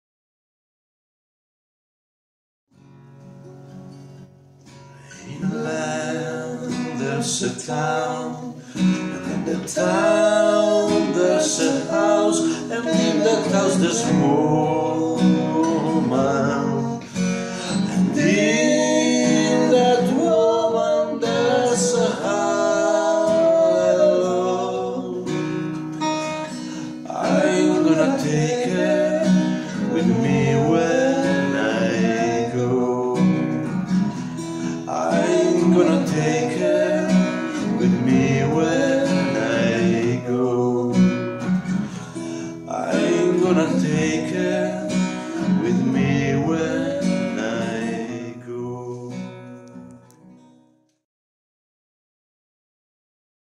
Animali Acustici: hai detto Tom Waits? - good ending
Animali Acustici è un duo musicale nato nel 2014 e composto da Massimiliano Cremona (voce, chitarra) e Gerardo Sempavor (voce, birretta).

Attualmente il repertorio di Animali Acustici comprende brani di: Alice in Chains, Simon and Garfunkel, Pearl Jam, Eddie Vedder, Afterhours, Radiohead, Depeche Mode, Rem, Neil Young, Patti Smith.

Come? Hai detto "Tom Waits"? Si, c'è anche lui. Good Ending.